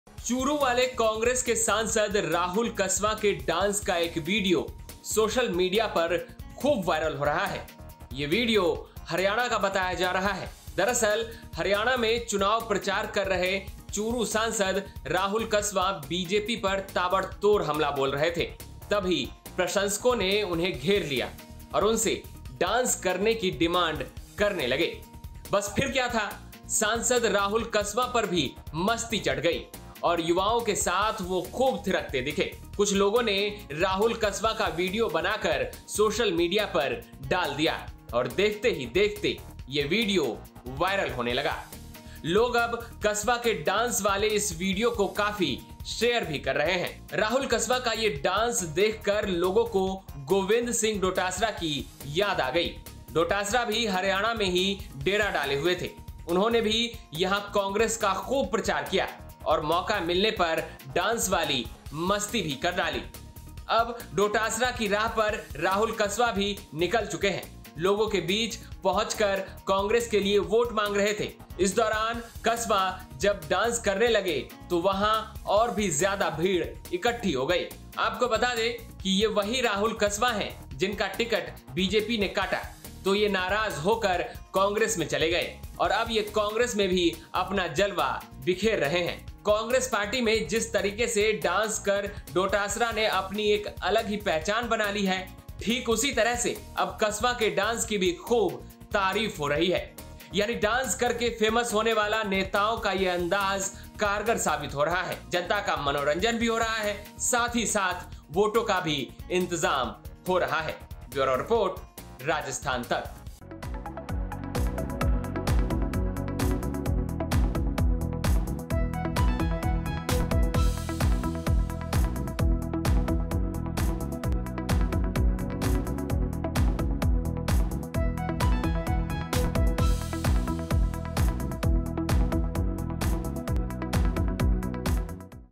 Haryana में नौजवानों संग Rahul Kaswan भी ख़ूब नाचे, डोटासरा की तरह फेमस हो गए!
चूरू वाले कांग्रेस के सांसद राहुल कस्वां के डांस का एक वीडियो सोशल मीडिया पर खूब वायरल हो रहा है। ये वीडियो हरियाणा का बताया जा रहा है।दरअसल हरियाणा में चुनाव प्रचार कर रहे चूरू सांसद राहुल कस्वां बीजेपी पर ताबड़तोड़ हमला बोल रहे थे, तभी प्रशंसकों ने उन्हें घेर लिया और उनसे डांस करने की डिमांड करने लगे, बस फिर क्या था, सांसद राहुल कस्वां पर भी मस्ती चढ़ गई और युवाओं के साथ वो खूब थिरकते दिखे। कुछ लोगों ने राहुल कस्वां का वीडियो बनाकर सोशल मीडिया पर डाल दिया और देखते ही देखते ये वीडियो वायरल होने लगा, लोग अब कस्वां के डांस वाले इस वीडियो को काफी शेयर भी कर रहे हैं। राहुल कस्वां का ये डांस देखकर लोगों को गोविंद सिंह डोटासरा की याद आ गई, डोटासरा भी हरियाणा में ही डेरा डाले हुए थे, उन्होंने भी यहां कांग्रेस का खूब प्रचार किया और मौका मिलने पर डांस वाली मस्ती भी कर डाली।